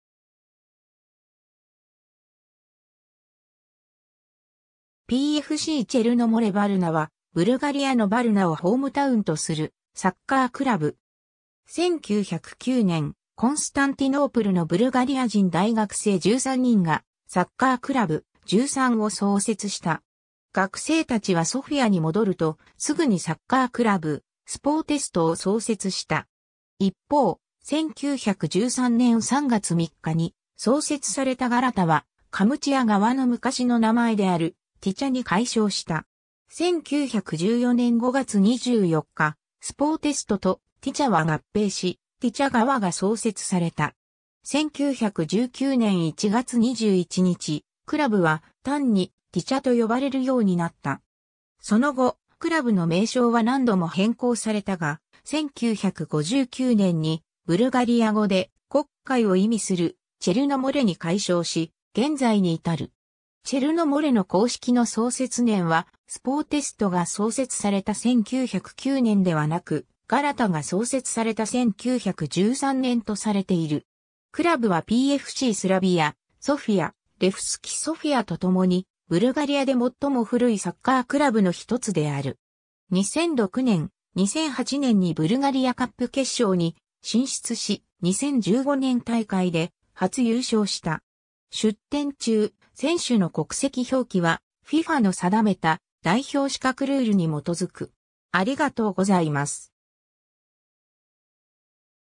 PFCチェルノ・モレ・ヴァルナ
ヴァルナ_(ブルガリア)
PFCチェルノ・モレ・ヴァルナ（ブルガリア語: ПФК Черно море Варна）は、ブルガリアのヴァルナをホームタウンとするサッカークラブ。
 1909年、コンスタンティノープルのブルガリア人大学生13人がサッカークラブ「13」を創設した。
学生たちはソフィアに戻ると、すぐにサッカークラブ「Sportist」（Спортист）を創設した。
一方、1913年3月3日に創設されたガラタはカムチヤ川の昔の名前であるティチャ（Тича）に改称した。
1914年5月24日、Sportistとティチャは合併し、ティチャ川（Река Тича）が創設された。
1919年1月21日、クラブは単にティチャと呼ばれるようになった。
 その後、クラブの名称は何度も変更されたが、1959年にブルガリア語で黒海を意味するチェルノ・モレに改称し、現在に至る。
 チェルノ・モレの公式の創設年はSportistが創設された1909年ではなく、ガラタが創設された1913年とされている。
クラブはPFCスラヴィア・ソフィア、レフスキ・ソフィアと共にブルガリアで最も古いサッカークラブの一つである。
 2006年、2008年にブルガリア・カップ決勝に進出し、2015年大会で初優勝した。
 出典 注：選手の国籍表記はFIFAの定めた代表資格ルールに基づく。